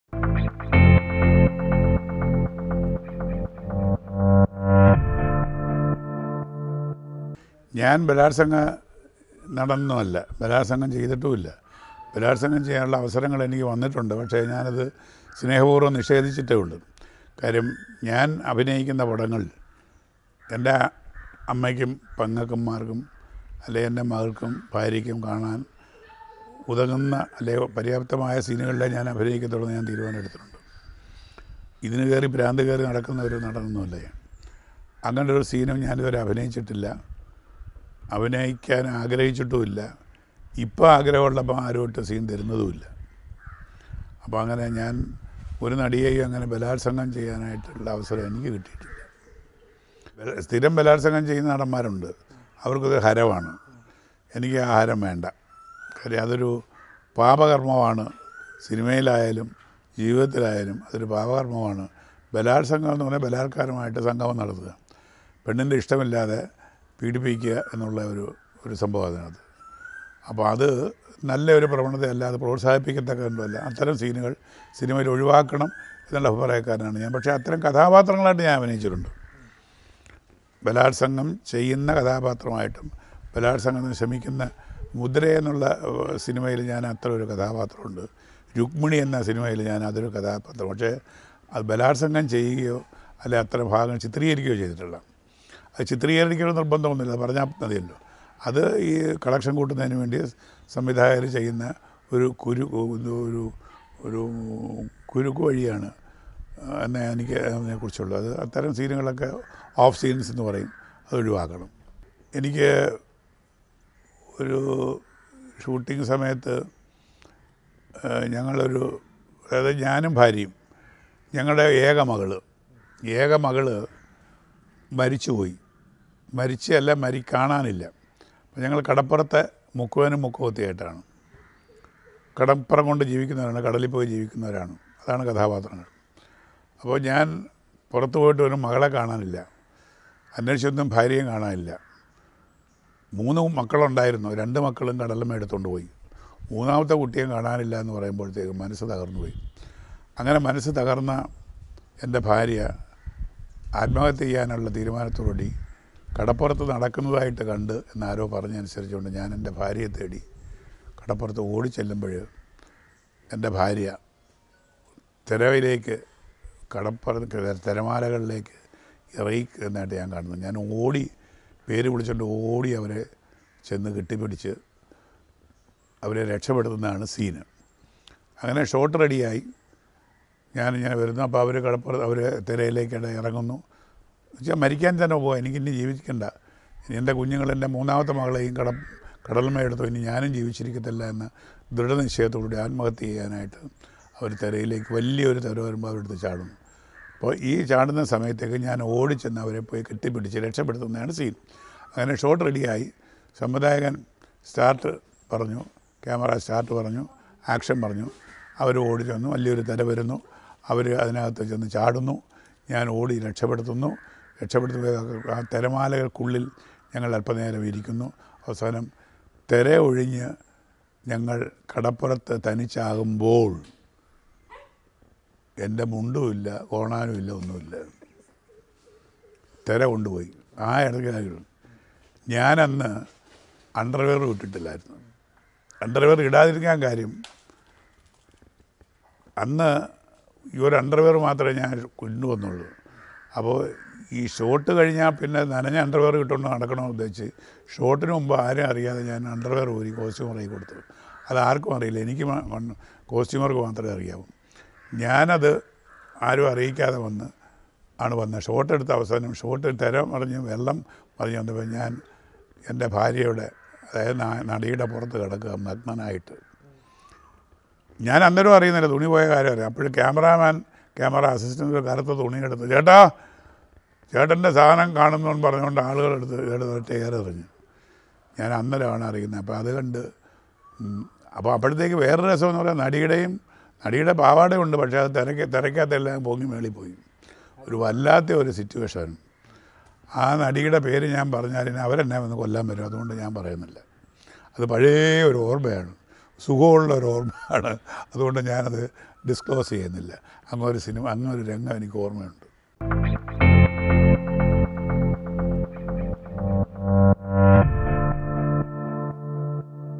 അടിവസ്ത്രം ഇല്ലാതെ അഭിനയിച്ച ഷോട്ട് കഴിഞ്ഞതും ഞാൻ മുഴുവനായി നടിയുടെ പുറത്ത്‌
മലയാള സിനിമ ആസ്ഥാന വില്ലന്മാരുടെ ലിസ്റ്റിലേക്ക് ഒതുക്കിയ നടനാണ് ശ്രീ.കൊല്ലം തുളസി..അദ്ദേഹത്തിന്റെ സിനിമ അനുഭവങ്ങളും അതിലെ ബുദ്ധിമുട്ടുകളെ പറ്റിയും സംസാരിക്കുകയാണ് അദ്ദേഹം..